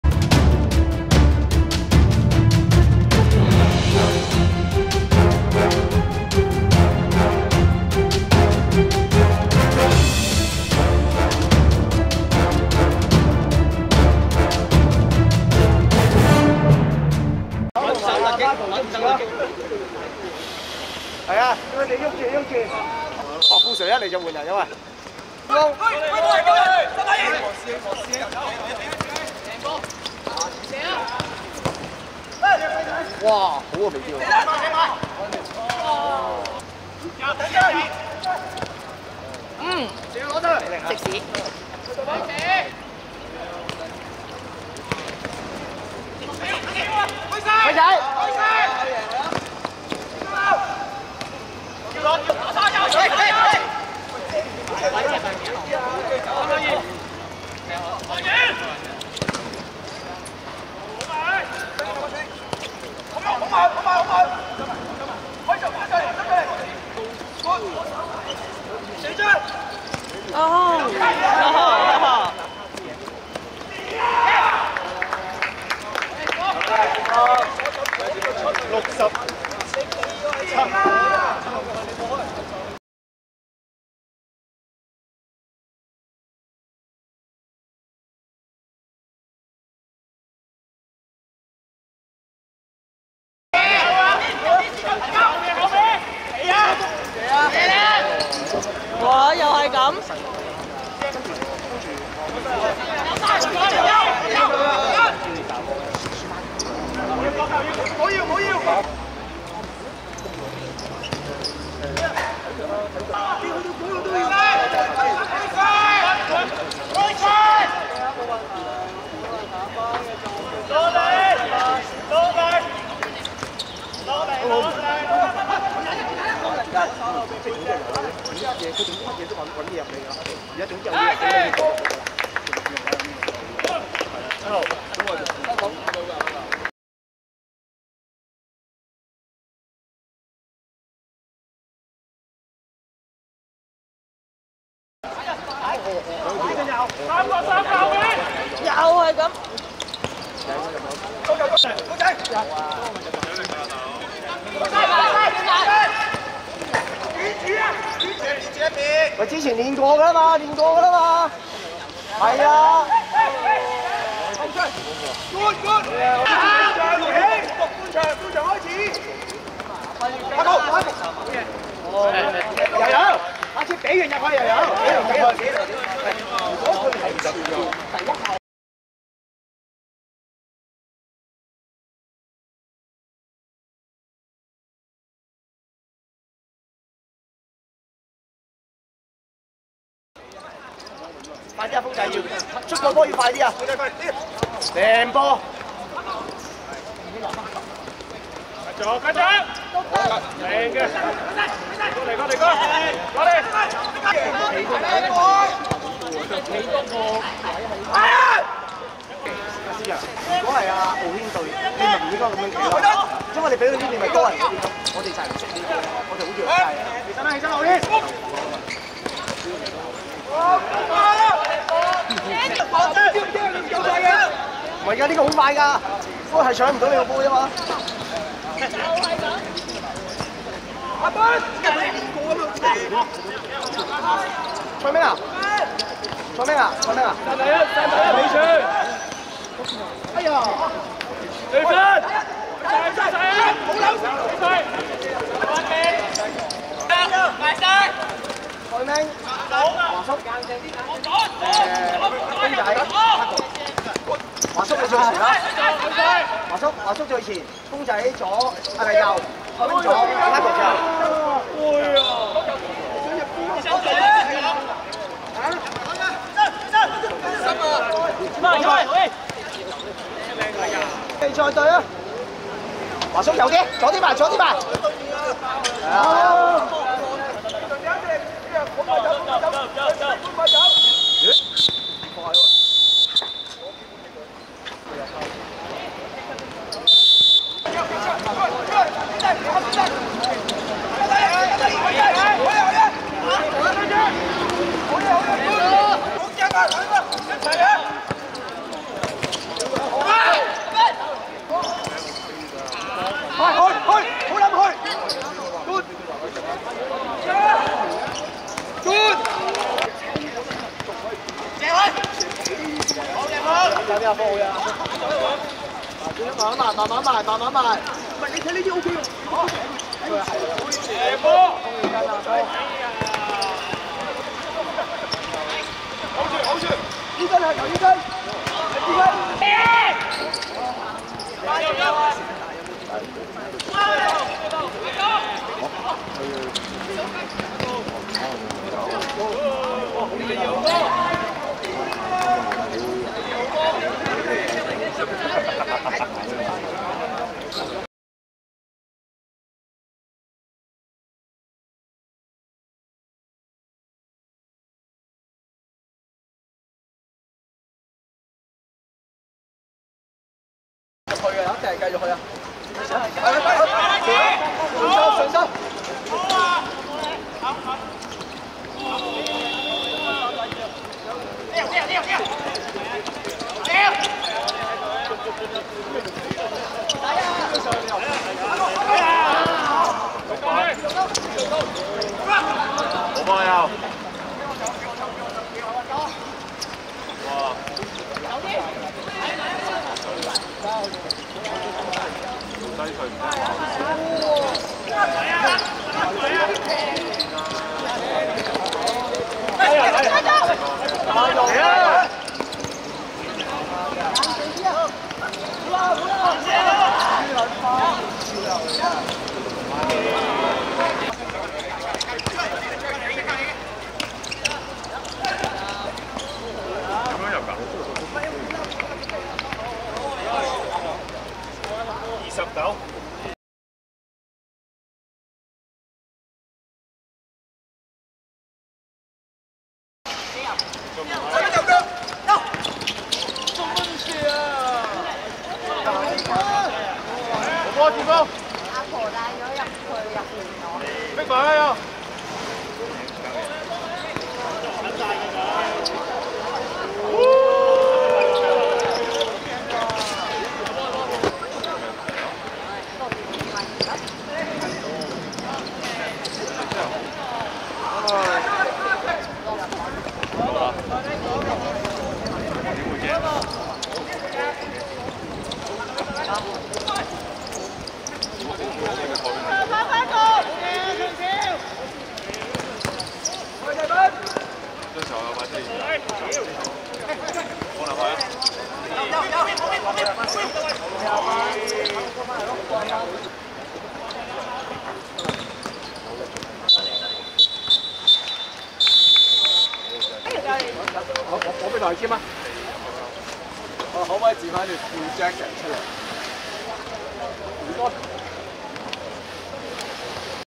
深水埗vs南區麗池(2021.7.24.賽馬會五人足球乙組聯賽~足總盃季軍賽)精華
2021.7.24.海心公園球塲 15:00
深水埗(綠) 0 : 5 南區麗池(黑)